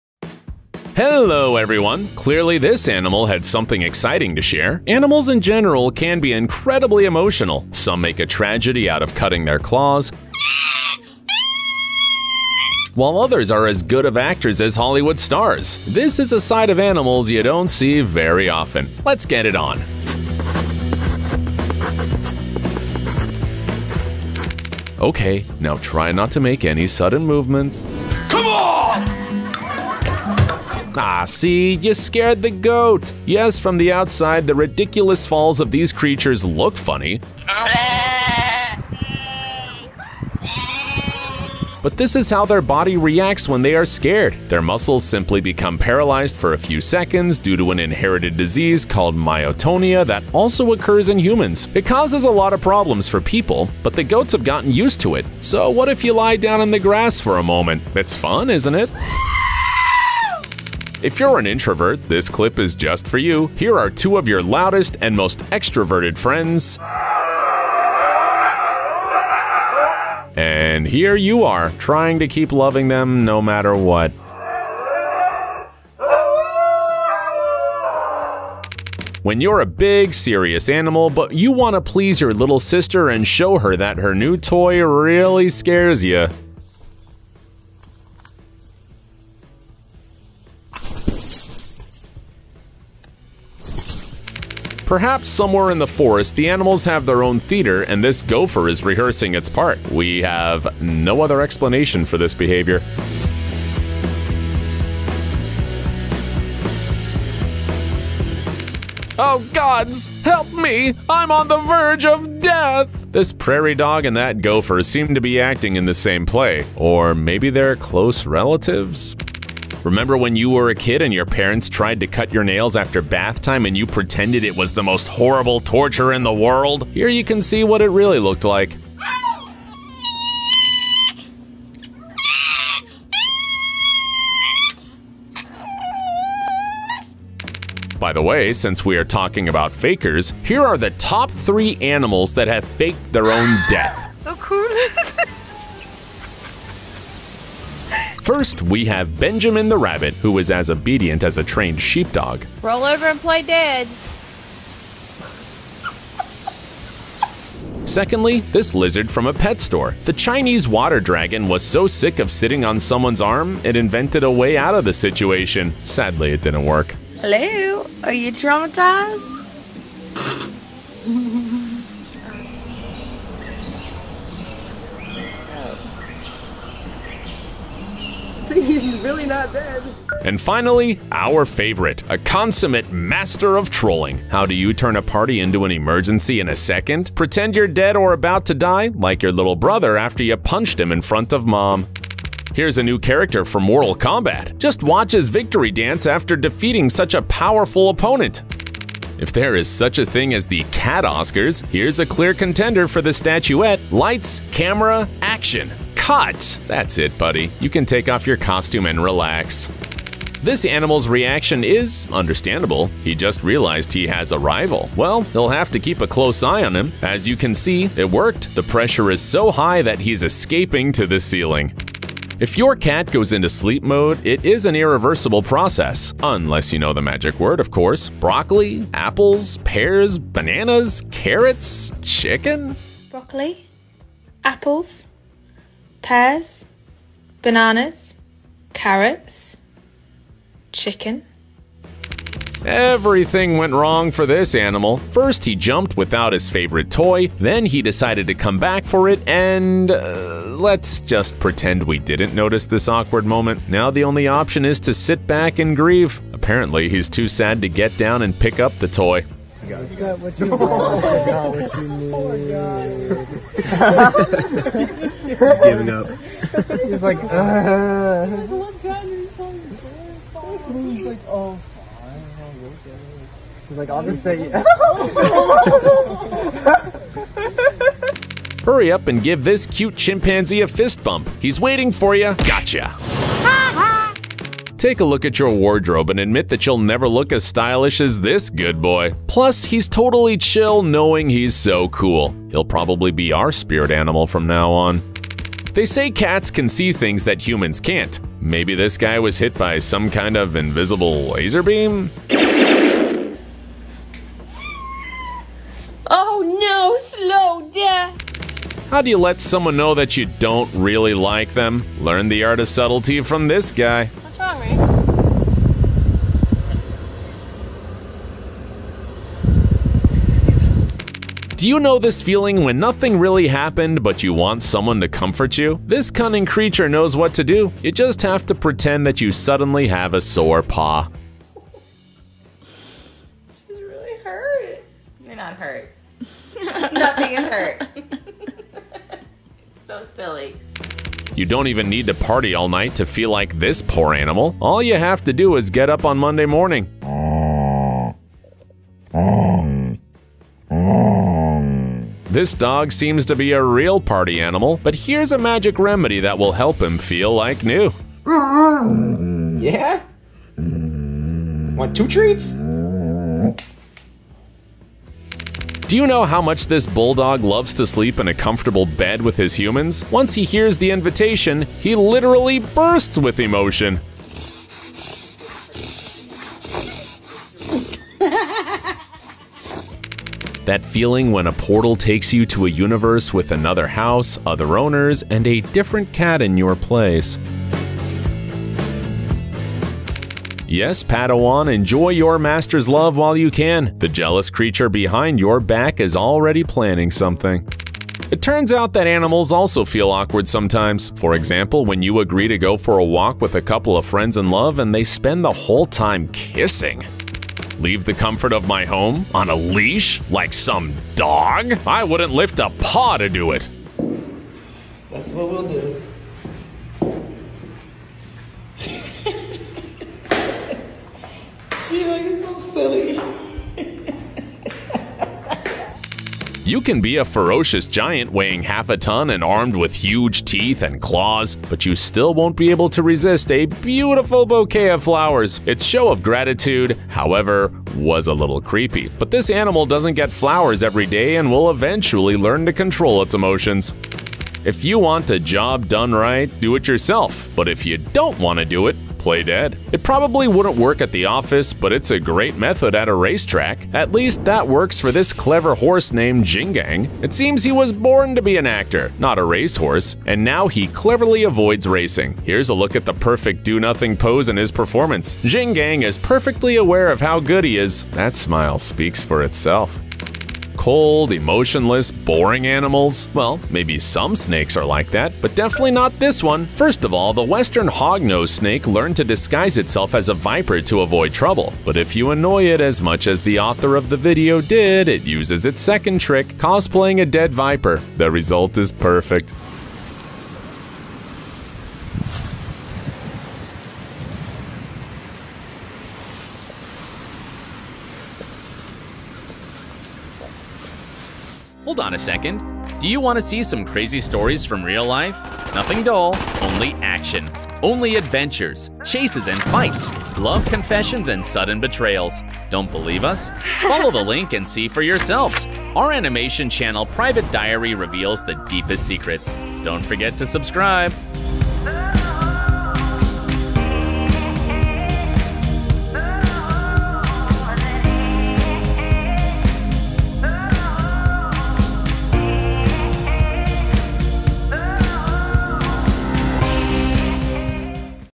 Overdramatic Pets That Will Make You Smile (2021)
Clearly this animal had something exciting to share; animals in general
can be incredibly emotional. Some make a tragedy out of cutting their
claws, while others are as good actors as Hollywood stars; this is a side of
animals you don't see very often.